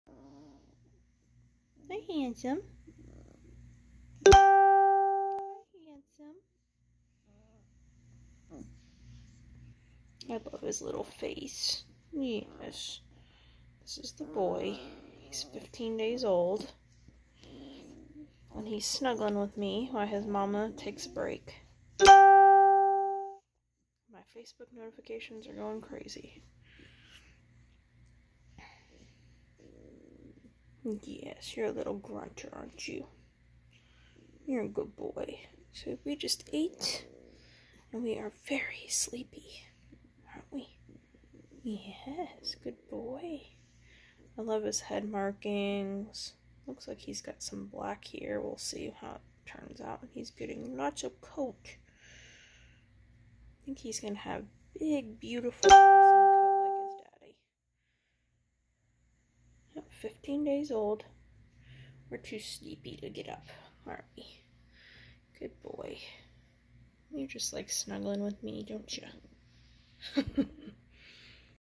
15 Day Old Varus
Our "League of Legends" Borzoi litter born July 9, 2020. The tri spotted is Vayne, the mostly white with brindle spots is Varus.